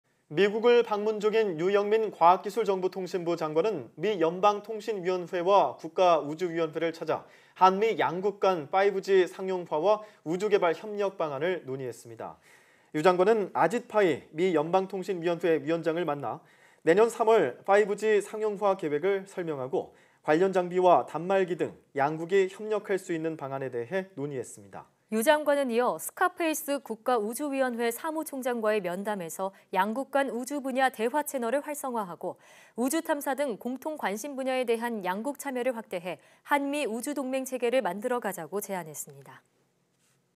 "5G·우주 개발 협력"…한·미, 방안 논의 / YTN 사이언스
미국을 방문 중인 유영민 과학기술정보통신부 장관은 미 연방통신위원회와 국가 우주위원회를 찾아 한미 양국 간 5G 상용화와 우주 개발 협력 방안을 논의했습니다.

유 장관은 아짓 파이 미 연방통신위원회 위원장을 만나 내년 3월 5G 상용화 계획을 설명하고 관련 장비와 단말기 등 양국이 협력할 수 있는 방안에 대해 논의했습니다.

유 장관은 이어 스캇 페이스 국가우주위원회 사무총장과의 면담에서 양국 간 우주 분야 대화 채널을 활성화 하고 우주탐사 등 공통 관심 분야에 대한 양국 참여를 확대해 한-미 우주 동맹 체계를 만들어 가자고 제안했습니다.